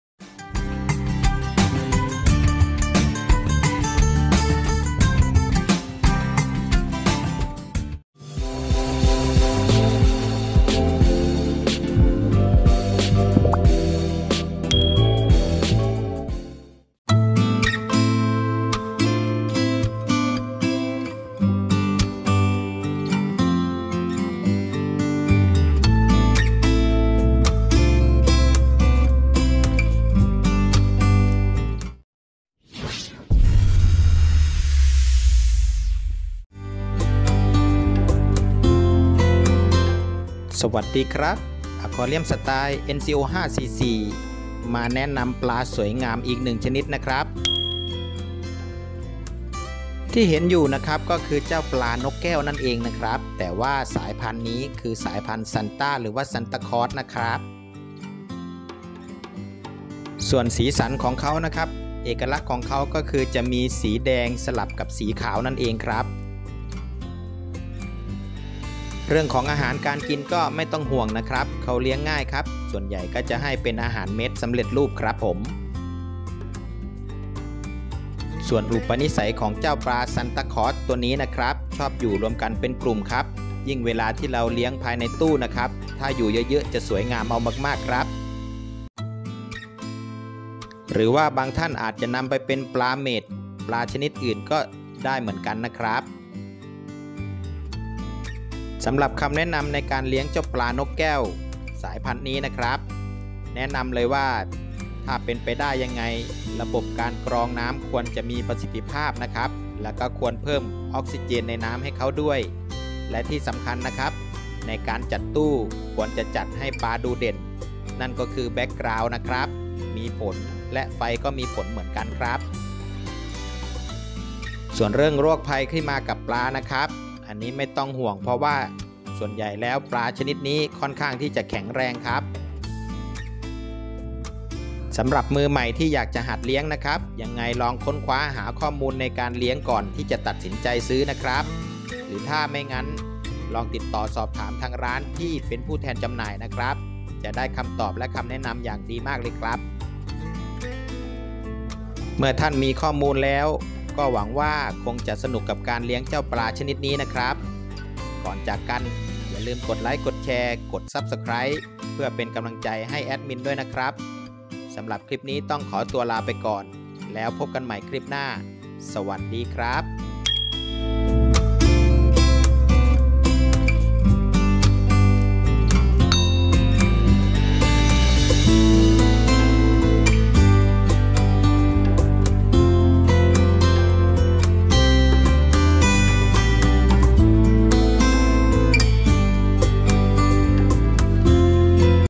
ปลานกแก้วซานต้า#NCO544
ปลานกแก้ว อีกหนึ่งชนิดที่ได้รับความนิยมในช่วงนี้ รูปร่าง สีสันสวยงาม เลี้ยงง่าย ราคาไม่แพง